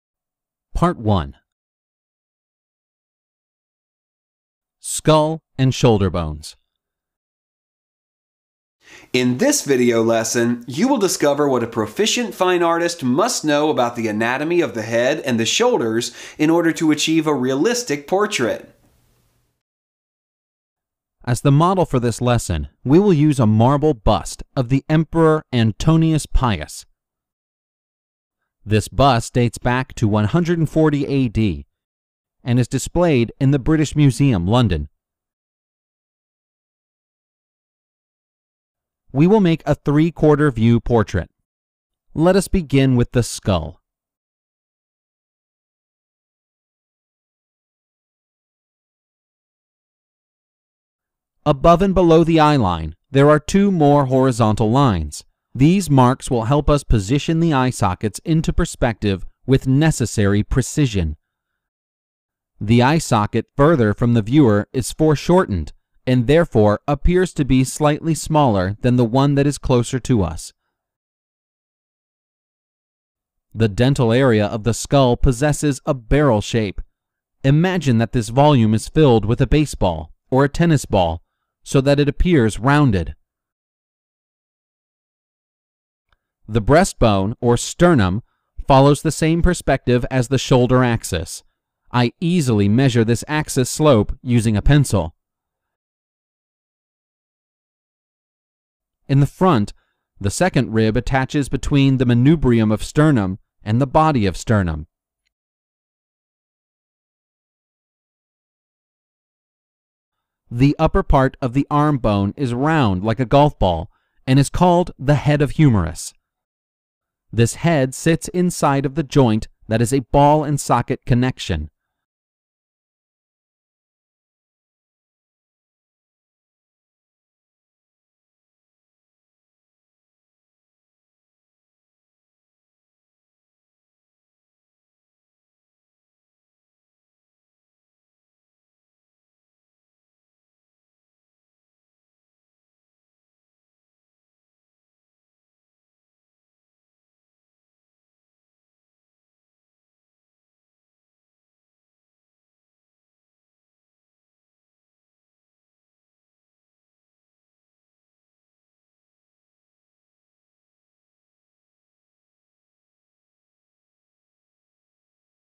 Head, Neck and Shoulders Skeletal Anatomy - Anatomy Master Class
- Discover Head, Neck and Shoulders Skeletal Anatomy - Anatomy Video Lesson for Figurative Artists